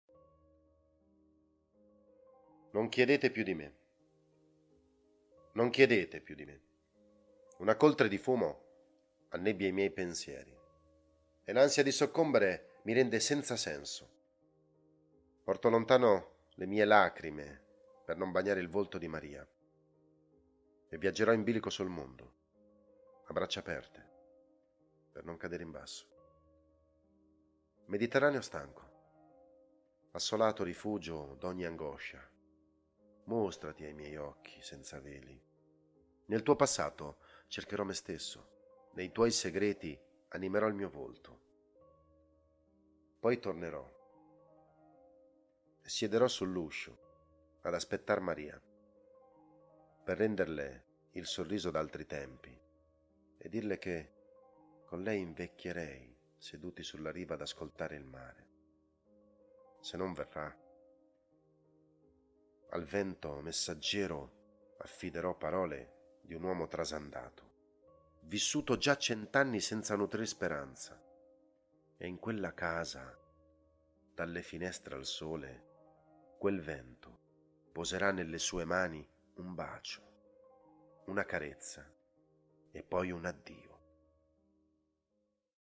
Carlo Petrone Non chiedetepiudime
(Potenza, 15/10/1946) Giornalista professionista, chitarrista appassionato di musica jazz e brasiliana, ha partecipato a programmi della Rai, Tv private e Radio private. Nel 2000 ha pubblicato un libro di poesie, Il soffio di un bacio, con prefazione di Giovanna Melandri. Scrivi poesie? Partecipa al Concorso. Primo Premio 1500€